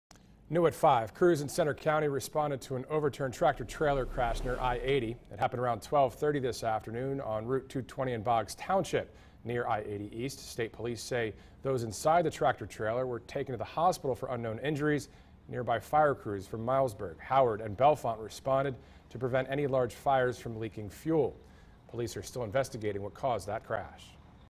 Tractor-trailer crashes, overturned on road in Centre County
Tractor-trailer crashes, overturned on road in Centre County 10-22-20